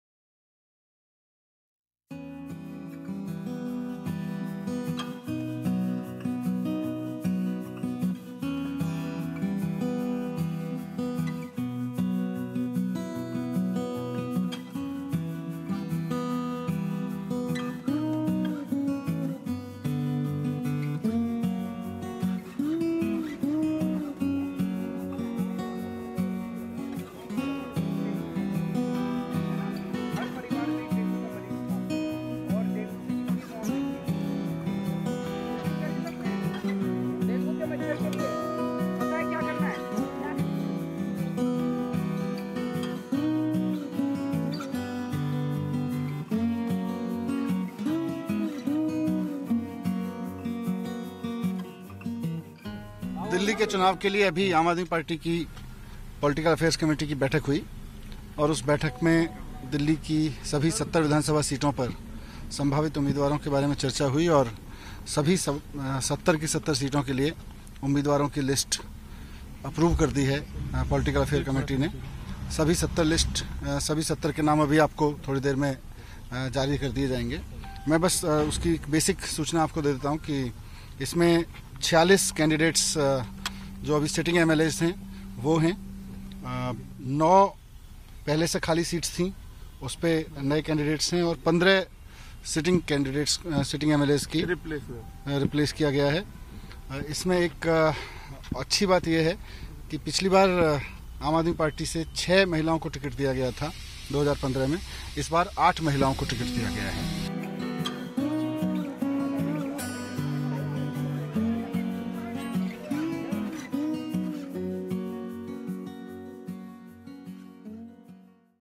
Delhi polls: AAP releases list of candidates for all 70 seats
New Delhi, Jan 14 (ANI): Aam Aadmi Party (AAP) released the list of candidates for the upcoming Delhi Assembly elections, on January 14. Chief Minister Arvind Kejriwal will contest from the New Delhi assembly constituency. Deputy CM Manish Sisodia will contest from Patparganj. Whereas, party leader Atishi, who was pitted against BJP’s Gautam Gambhir in Lok Sabha elections, will contest the Kalkaji seat.